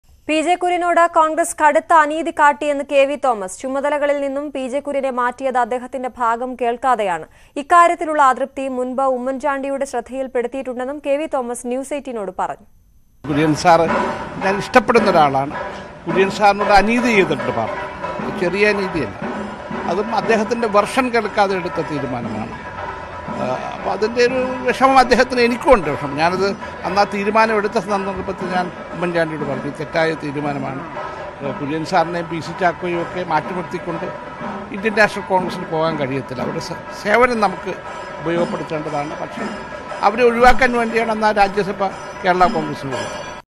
''P. J. Kurien സാറിനോട് കടുത്ത അനീതിയാണ് Congress പാർട്ടി ചെയ്‌തത്‌'': K V Thomas
മുതിർന്ന Congress നേതാവ് P. J. Kurien കടുത്ത അനീതിയാണ് Congress പാർട്ടി ചെയ്‌തതെന്ന് K V Thomas മാധ്യമങ്ങളോട് പറഞ്ഞു. ചുമതലകളിൽ നിന്നും അദ്ദേഹത്തെ മാറ്റിയത് അദ്ദേഹത്തിന്റെ ഭാഗം കേൾക്കാതെയാണെന്നും K V Thomas മാധ്യമങ്ങളോട് പറഞ്ഞു.